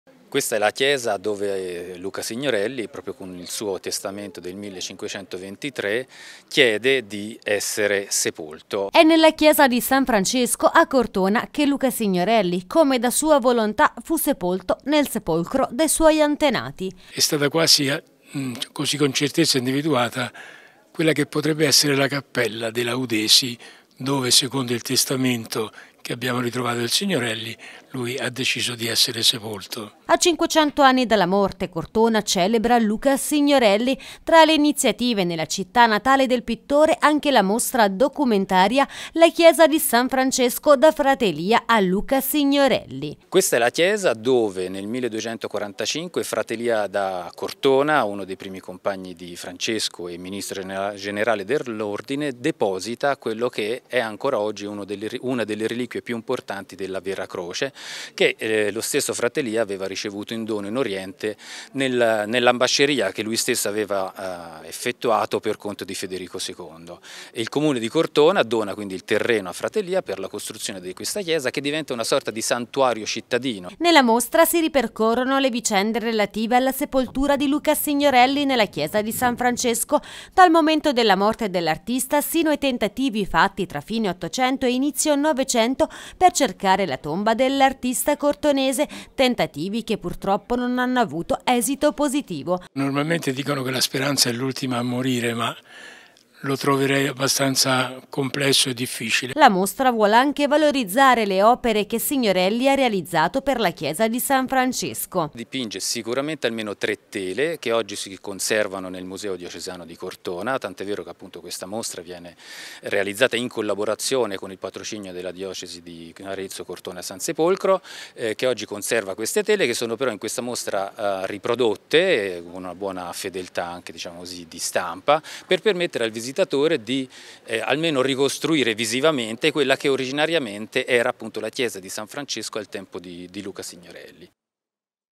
A Cortona la mostra "La chiesa di San Francesco da frate Elia a Signorelli"
A 500 anni dalla morte Cortona celebra Luca Signorelli.
Tra le iniziativa nella città natale del pittore  anche la mostra documentaria "In sepulchro suorum antiquorum. La chiesa di San Francesco da frate Elia a Luca Signorelli", promossa dal Centro studi frate Elia da Cortona, in collaborazione col Museo diocesano di Cortona.
Proprio in questa chiesta Luca Signorelli, come da sua volontà, fu sepolto, nel sepolcro dei suoi antenati.